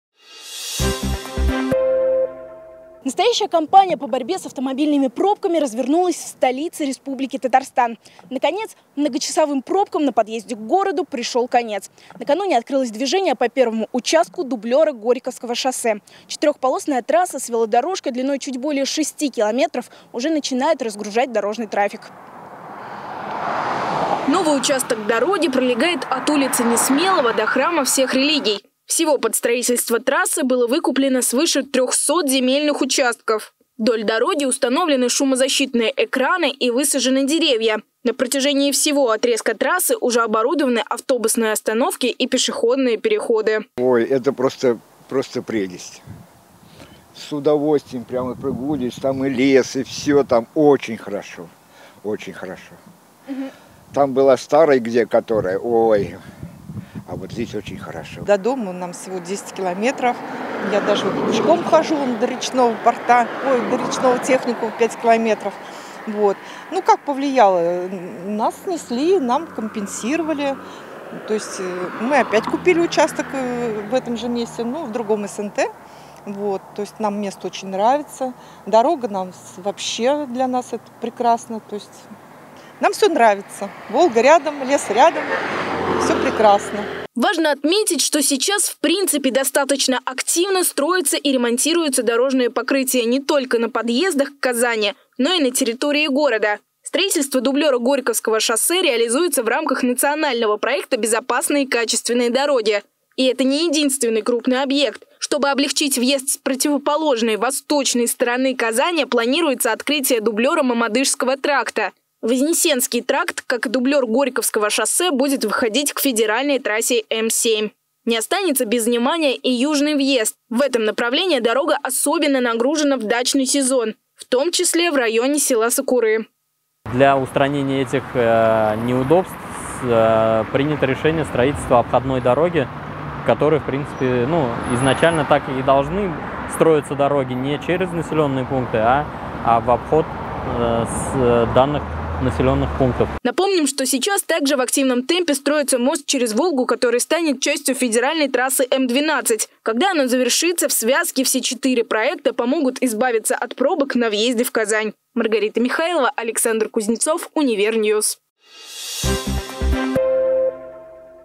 ДУБЛЁР ГОРЬКОВСКОГО ШОССЕ
Накануне в столице Татарстана открылось движение по участку дублёра Горьковского шоссе. В чём преимущества новой трассы - расскажем в сюжете